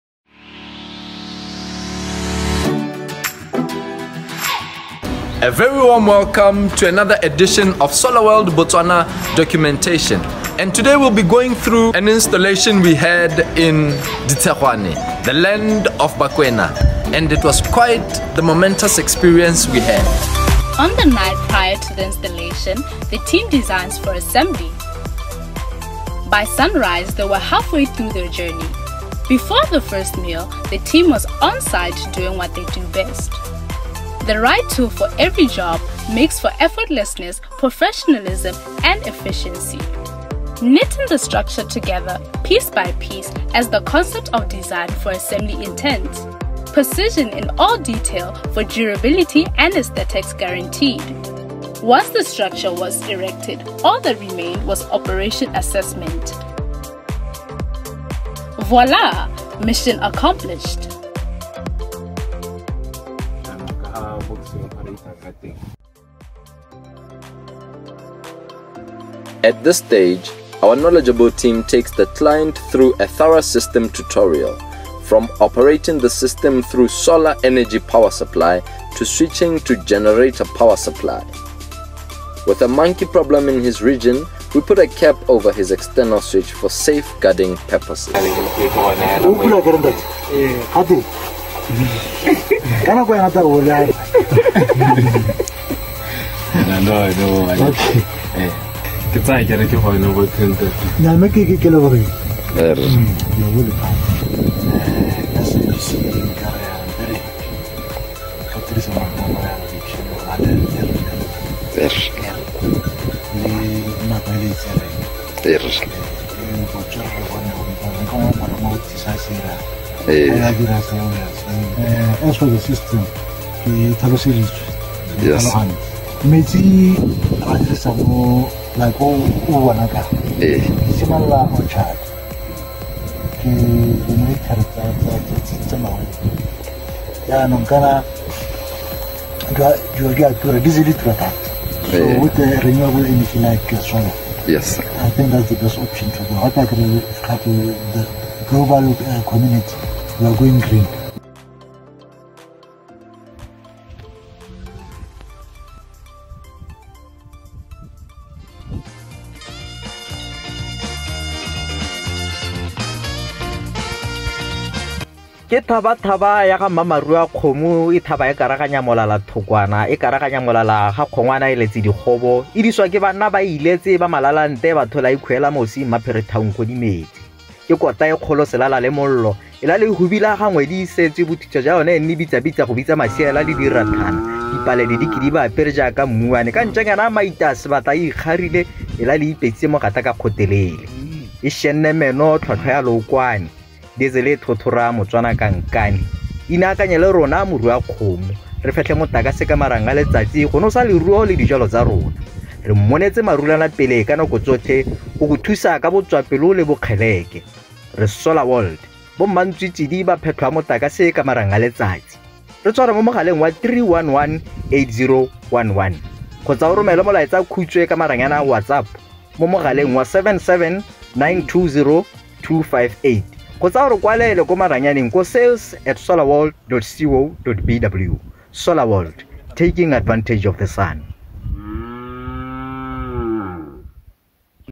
Solar Borehole System
Botswana’s current energy, fuel and diesel situation(s) have rendered the agricultural sector and market into a rather unenviable subservience to the merciless clenches of trade and importation of resources, but Solar World Botswana is proud to present to you the solution that will without a doubt unburden the nation of this tyranny, the future of energy is renewable.

With that said, we hope you enjoy watching our Ditshegwane-project documentary film, half as much as we enjoyed making it, because we had a Ball!